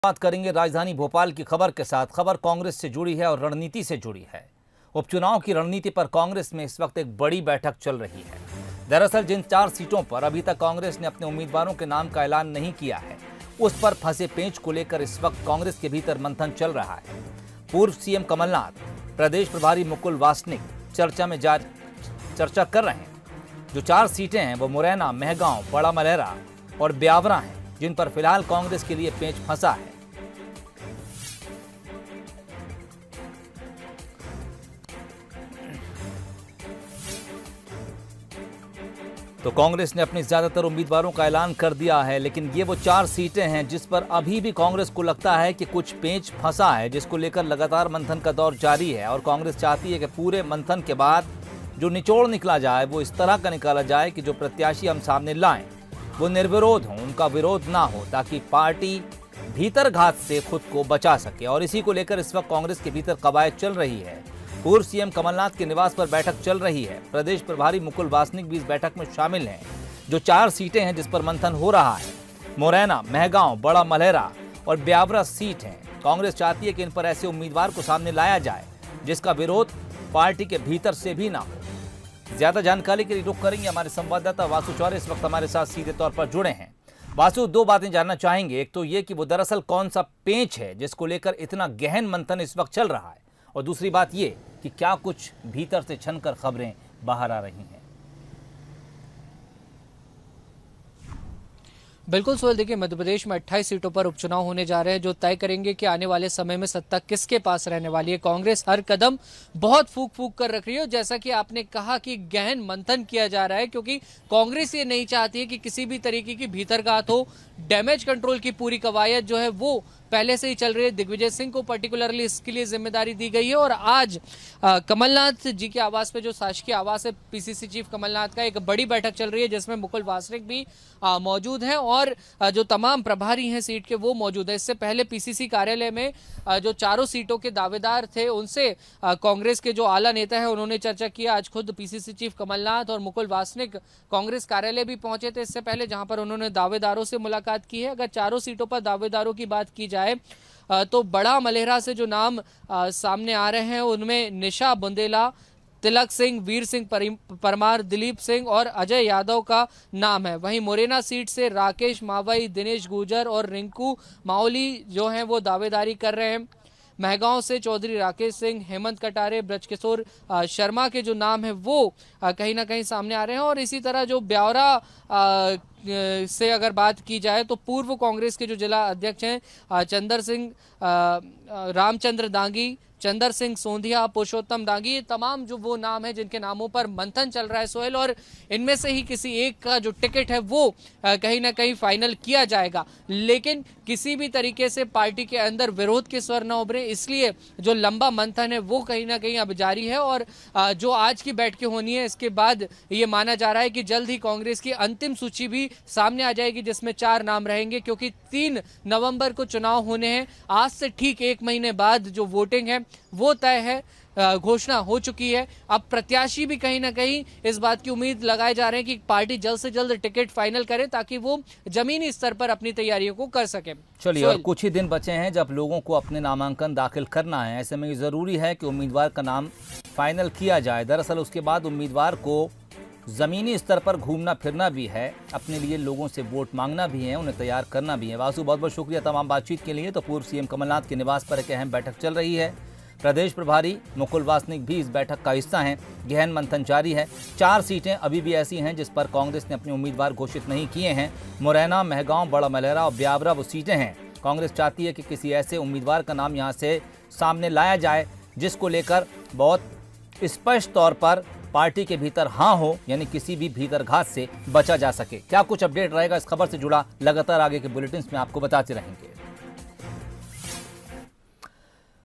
उपचुनाव की रणनीति पर कांग्रेस की बड़ी बैठक, फंसी 4 सीटों पर मंथन !
मध्यप्रदेश मे होने वाले  उपचुनाव के लिए सभी दल अपनी अपनी रणनीती बना रहे है. इसी कड़ी में पीसीसी अध्यक्ष कमलनाथ के निवास पर कांग्रेस की बैठक जारी है. इस बैठक में 4 सीटों पर फंसे पेंच को लेकर मंथन चल रहा. दरअसल मुरैना, मेहगांव, बड़ा मलहरा और ब्यावरा सीट पर कांग्रेस अपने प्रत्याशियों का ऐलान अभी तक नहीं कर पाई है.